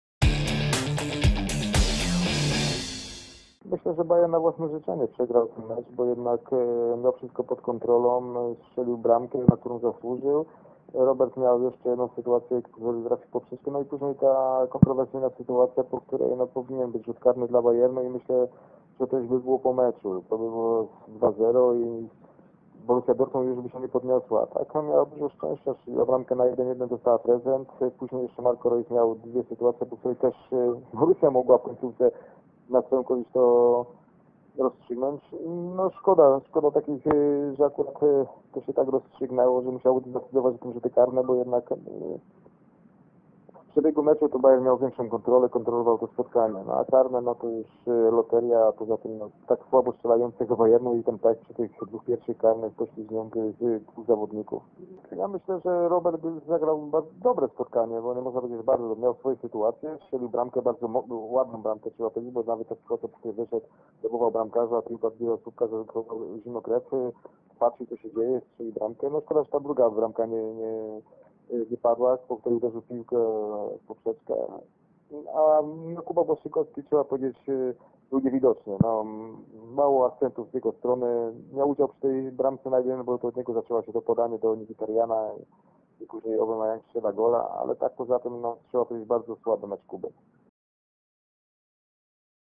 Puchar Niemiec Bayern - Borussia. Chałaśkiewicz: Powinien być rzut karny dla Bayernu
Borussia Dortmund została pierwszym finalistą Pucharu Niemiec. Podopieczni Jurgena Kloppa pokonali po rzutach karnych na Alianz Arenie Bayern Monachium. W regulaminowym czasie gry był remis 1:1. - Bayern przegrał to spotkanie na własne życzenie, bo miał mecz pod kontrolą - mówi Sławomir Chałaśkiewicz, były piłkarz Bundesligi.